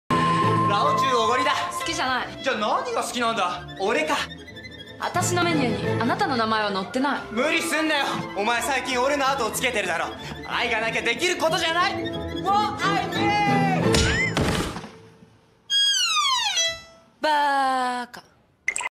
Otoya flirting fail
Otoya's most hilarious attempt at getting Yuri.